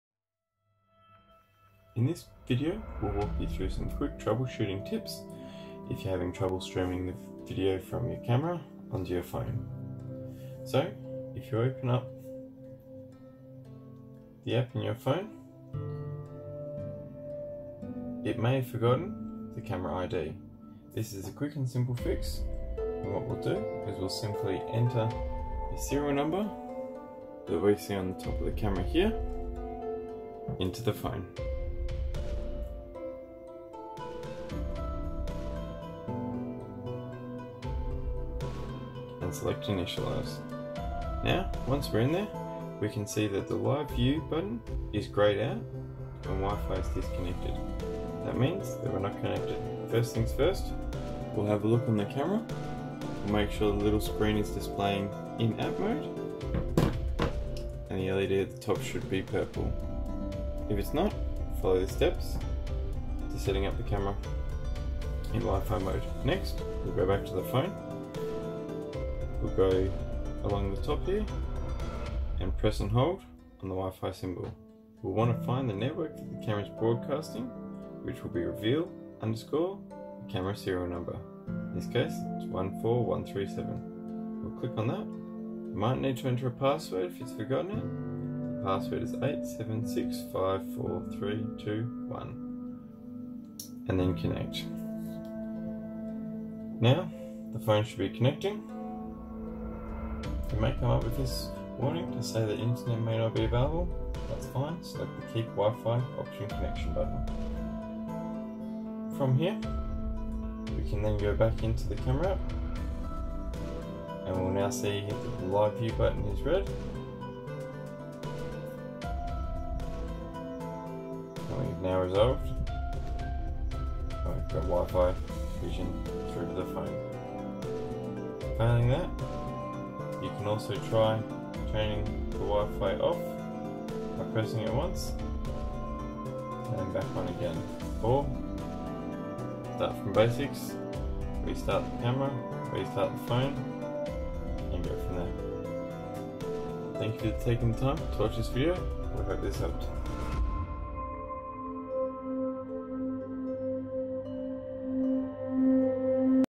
RVSU Troubleshooting Camera Feed
This is a short troubleshooting steps in the unlikely event that there is no feed from the camera to the phone.